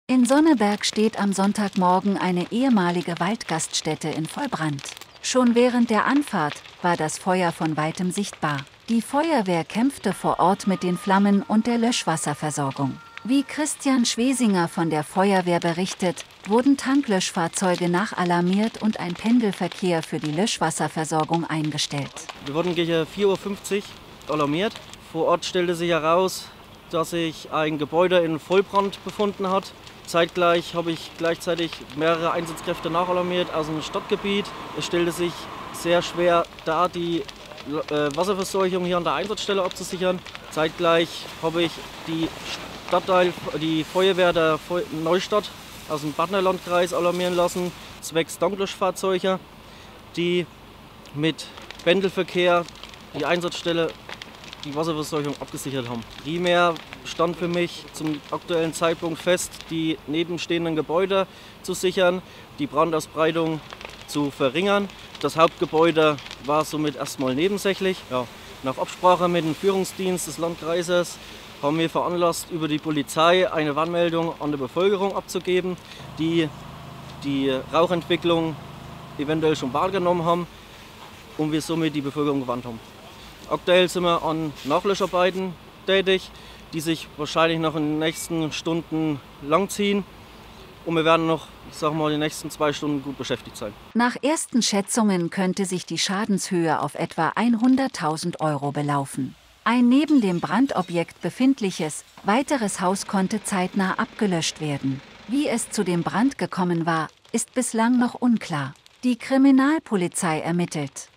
Flammen schlagen aus ehemaliger Gaststätte "Waldhaus"
In Sonneberg steht am Sonntagmorgen (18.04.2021) eine ehemalige Waldgaststätte in Vollbrand. Schon während der Anfahrt, war das Feuer von weitem sichtbar. Die Feuerwehr kämpfte vor Ort mit den Flammen und der Löschwasserversorgung. Wie Christian Schwesinger von der Feuerwehr berichtet, wurden Tanklöschfahrzeuge nachalarmiert und ein Pendelverkehr für die Löschwasserversorgung eingestellt. Laut Informationen von vor Ort galt es hauptsächlich die nebenstehenden Gebäude zu sichern und das Hauptgebäude kontrolliert abbrennen zu lassen. Wegen der starken Rauchentwicklung wurde eine Warnmeldung an die Bevölkerung herausgegeben. Bei den Löscharbeiten verletzte sich einer der Feuerwehrmänner leicht und wurde in ein Krankenhaus gebracht. Nach ersten Schätzungen könnte sich die Schadenshöhe auf etwa 100.000 Euro belaufen. Ein neben dem Brandobjekt befindliches, weiteres Haus konnte zeitnah abgelöscht werden. Wie es zu dem Brand gekommen war, ist bislang noch unklar. Die Kriminalpolizei ermittelt.

Das "Waldhaus" wurde früher als Ausflugsgaststätte betrieben. Seit Jahren wurde die ehemalige Gaststätte laut Verfassungsschutzbericht aus 2018 hauptsächlich als Veranstaltungsörtlichkeit und Treffpunkt der rechtsextremen Szene aus Südthüringen genutzt.